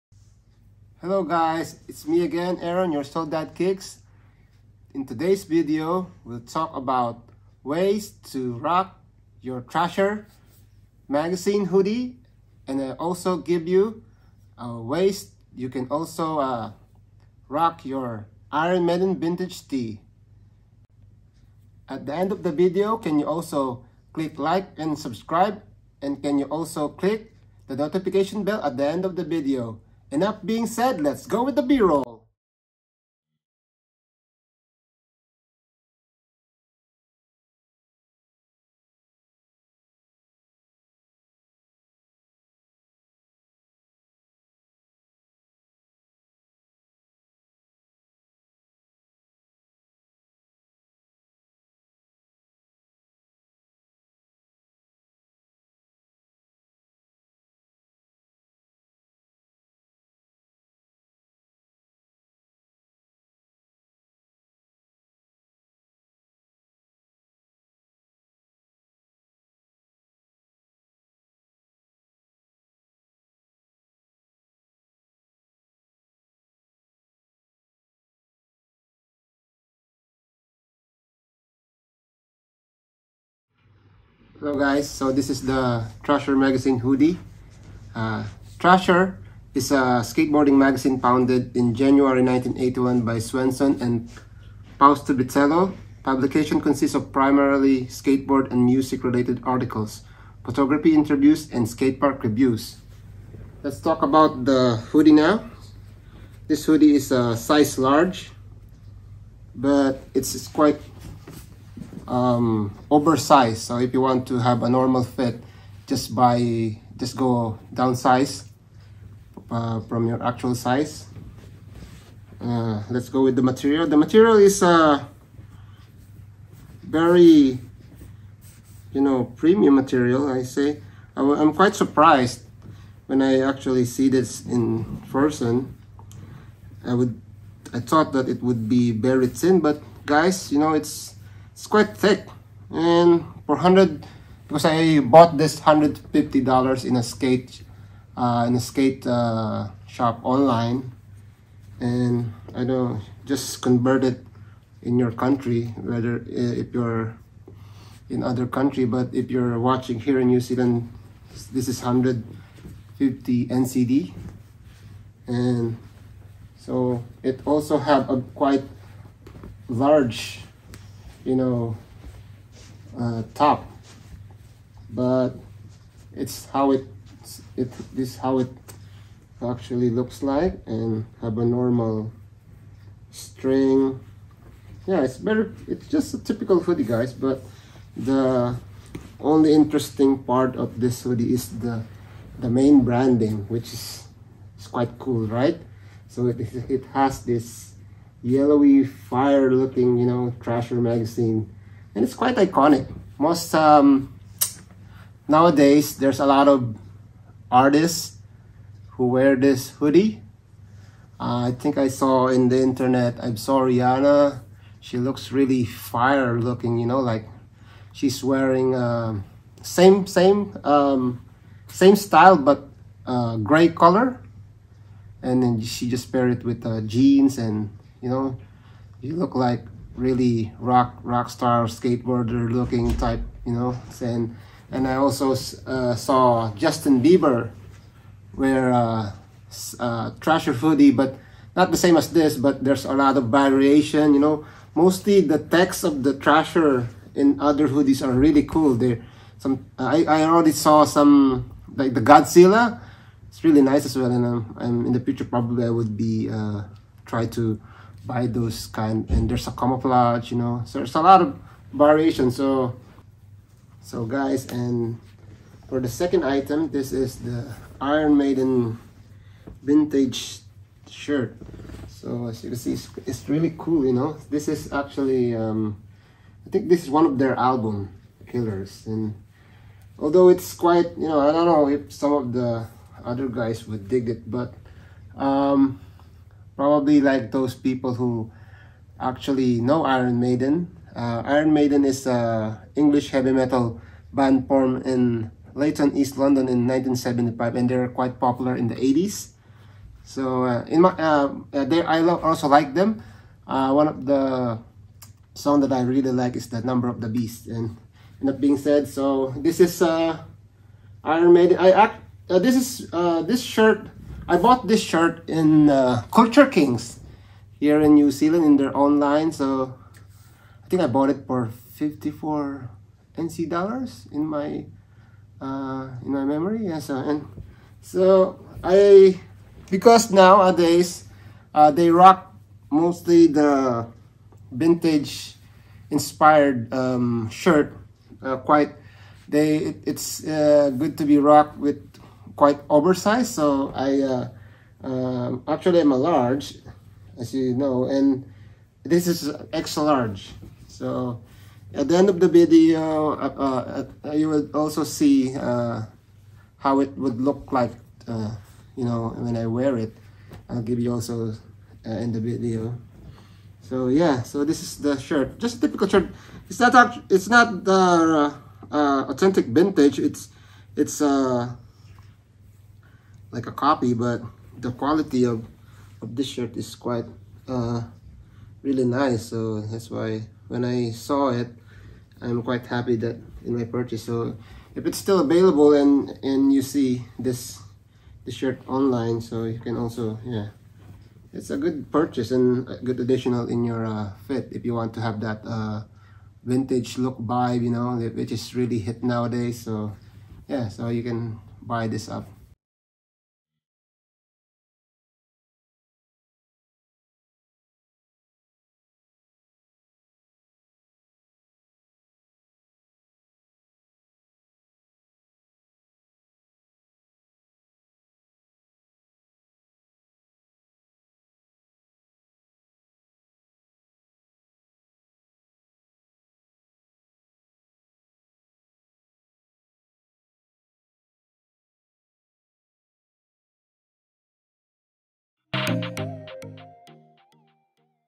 Styles ideas for Thrasher and Iron Maiden tee
Hello guys today we're gonna show you ideas for Thrasher and Iron Maiden tee!!!